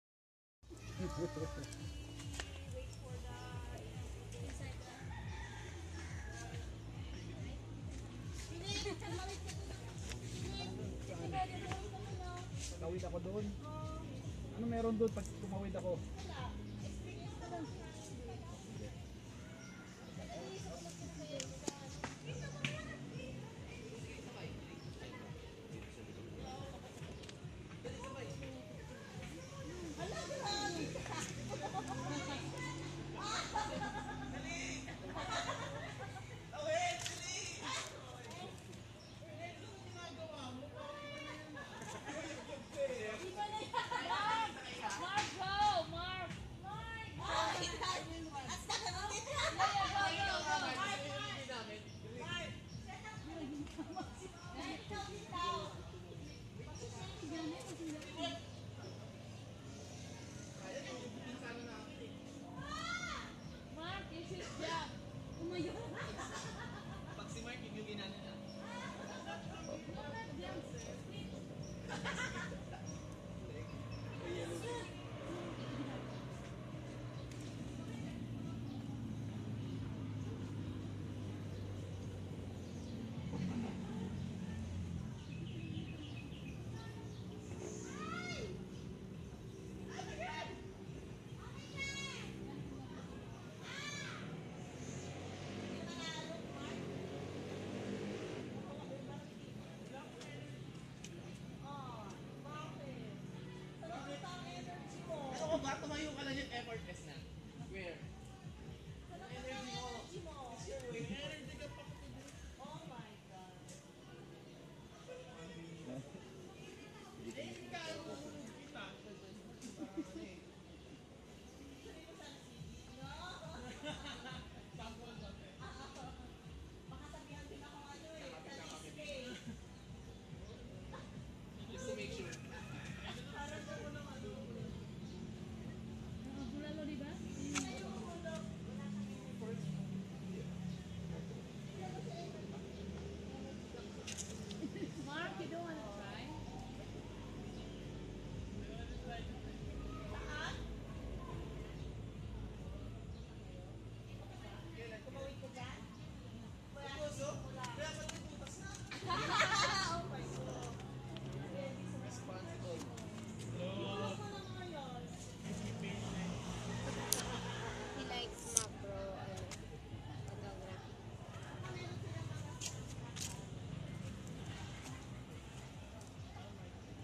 Follow the black ant - ตามรอยมด
While I was in Pranjetto Hills, I want to know where this ant could go so I tried to follow it.

ลองตามรอยมดดำตัวใหญ่ดูเล่นๆครับ ผลออกมาคือ.